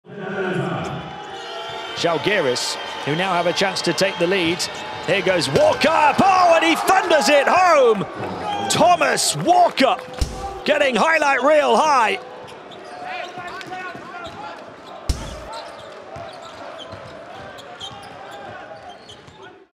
7DAYS Magic Moment of the Night: Thomas Walkup, Zalgiris Kaunas
Thomas Walkup gives Zalgiris the lead in stunning style with a ferocious slam!